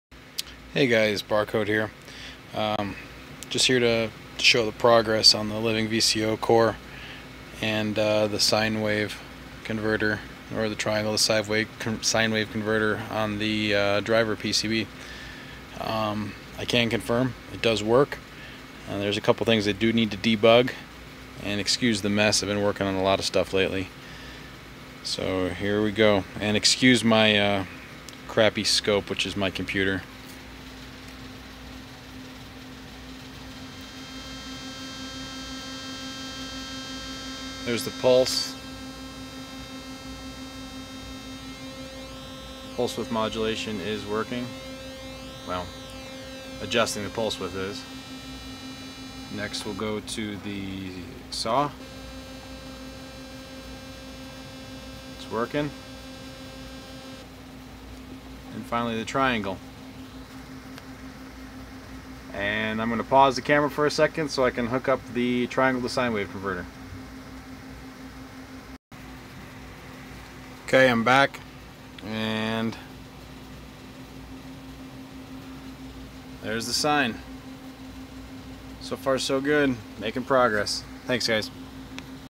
Living VCO First Test
Here's the first test of the J. Haible Living VCO.